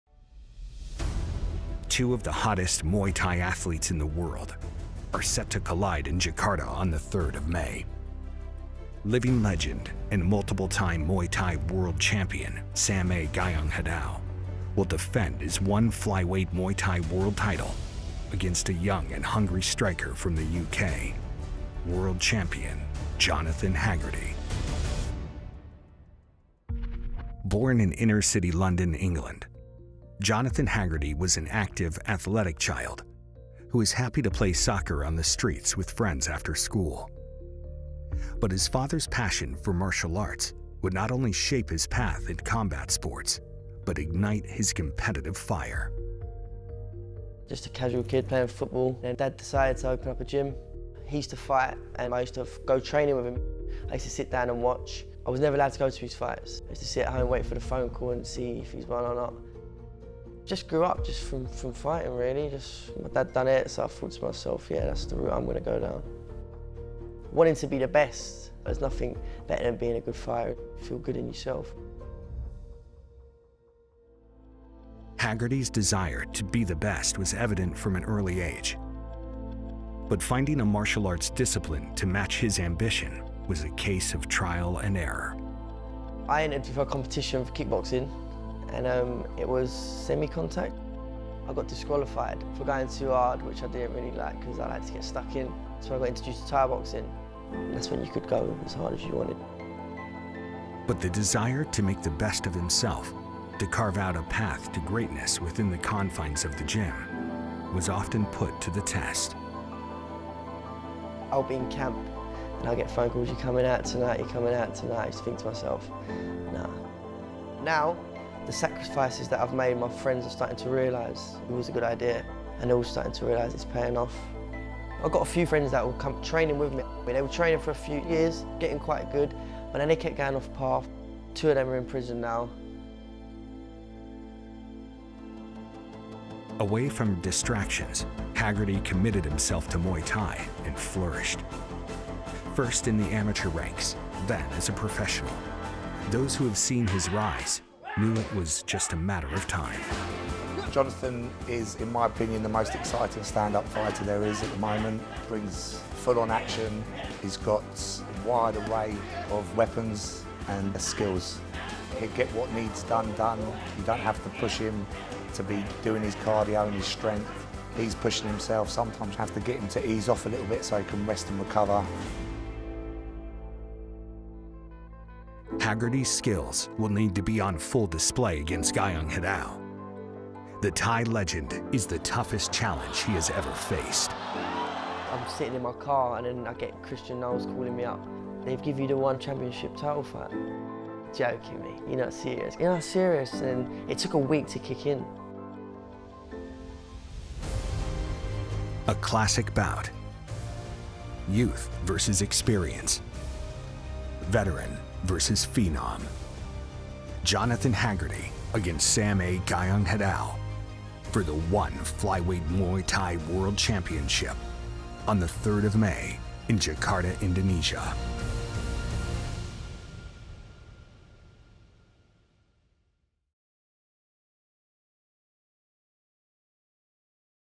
Jonathan Haggerty's Unwavering Devotion To Muay Thai | ONE Special Feature
Jonathan Haggerty’s innate talent was evident from a young age. Following in his dad’s footsteps, Jonathan Haggerty competed in amateur tournaments in his younger days winning multiple accolades before turning professional. Through all the success he experienced, he knew that a career in Muay Thai was his destiny and dedicated his life to the sport. That incredible level of commitment has now brought him to the cusp of greatness as he has earned a shot at the ONE Flyweight Muay Thai World Title at ONE: FOR HONOR in Jakarta.